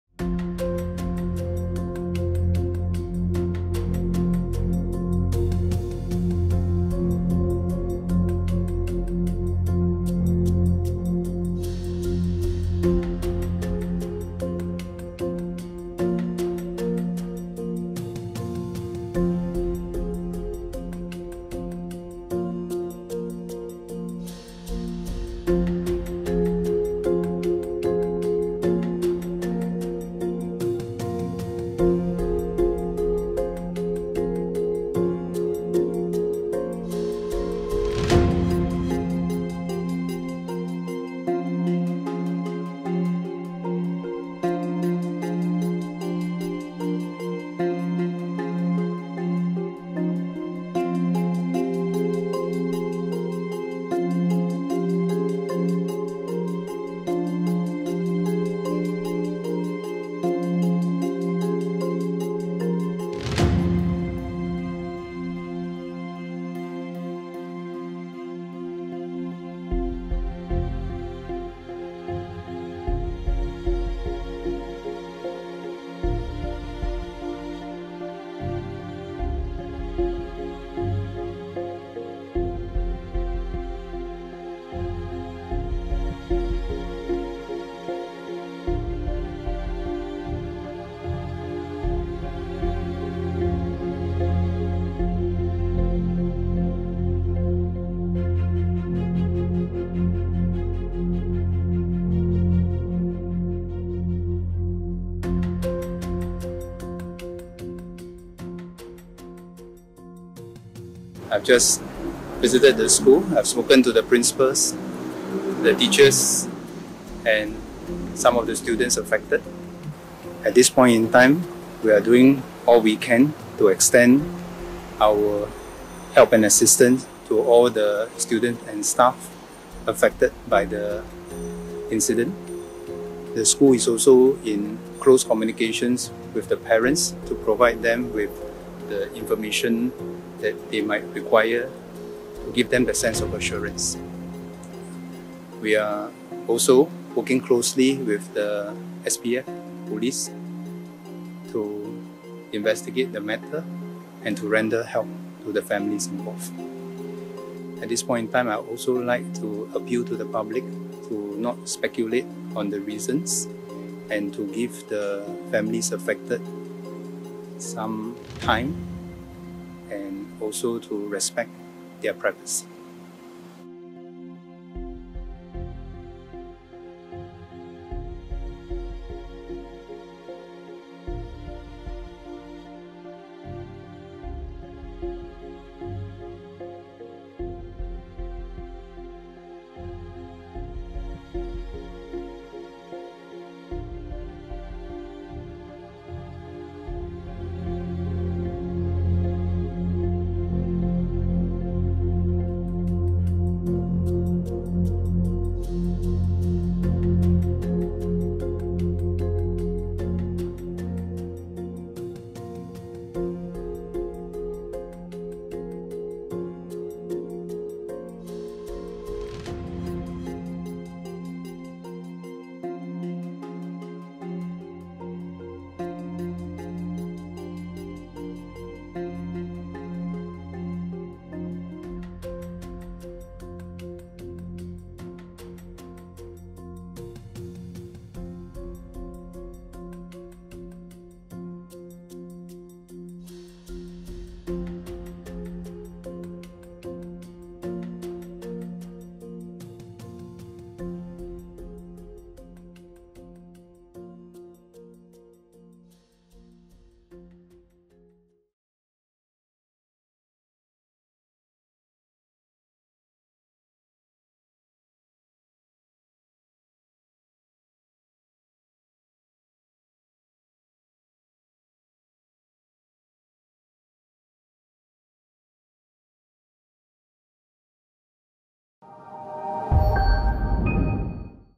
River Valley High School death: A timeline
A 13-year-old Secondary 1 student of River Valley High School was killed on campus on July 19, allegedly by a 16-year-old Secondary 4 student. Read the story